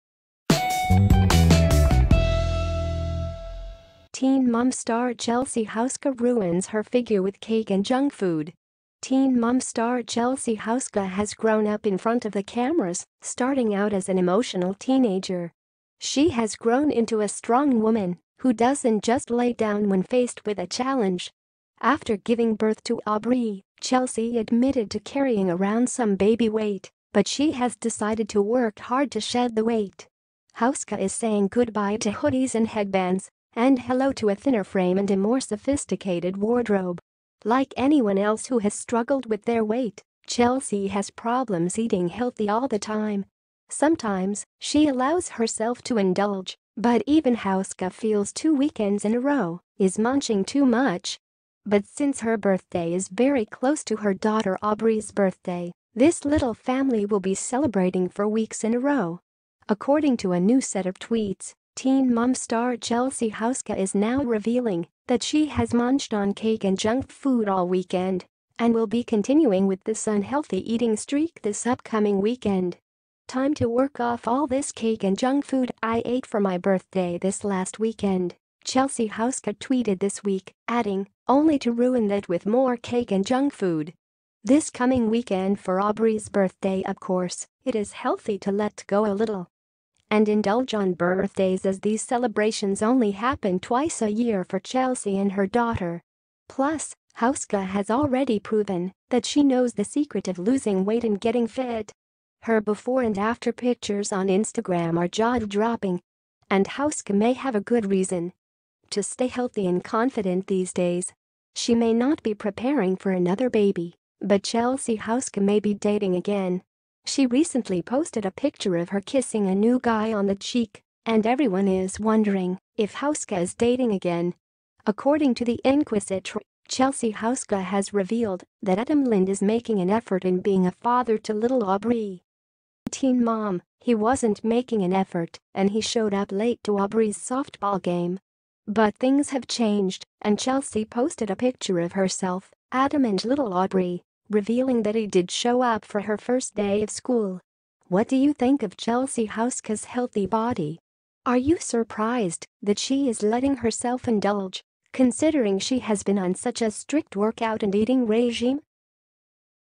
Teen Mom Star Chelsea Houska Ruins Her Figure With Cake and Junk Food?
Teen Mom star Chelsea Houska has grown up in front of the cameras, starting out as an emotional teenager. She has grown into a strong woman, who doesn’t just lay down when faced with a challenge. After giving birth to Aubree, Chelsea admitted to carrying around some baby weight, but she has decided to work hard to shed the weight. Houska is saying goodbye to hoodies and headbands, and hello to a thinner frame and a more sophisticated wardrobe.